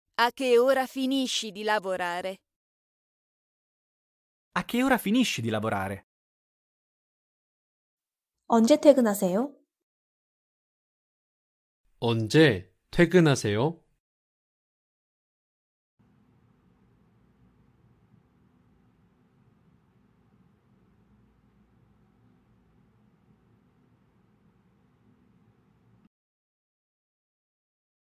이탈리아어로 "언제 퇴근하세요?" 말하는 법
남자 목소리 :
A che ora finisci di lavorare?

남성 음성 번역 :
언제 퇴근하세요?

여성 음성 번역 :
언제 퇴근하세요?